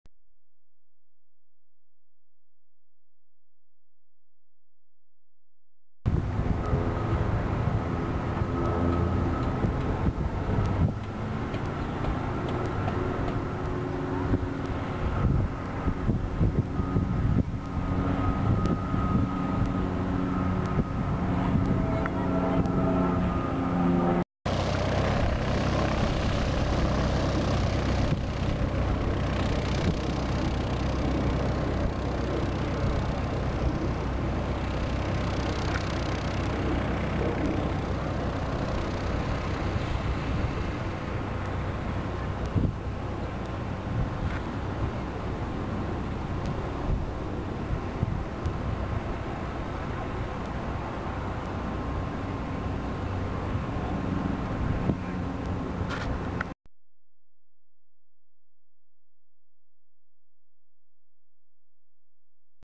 ANOTHER BLIMP - HANGAR STYLE! - Frederick, MD
WOW! Another Blimp Visit @ Frederick, Maryland Airport!  We have had GOODYEAR, MET LIFE and now the HANGAR Blimp ---all in about a months time.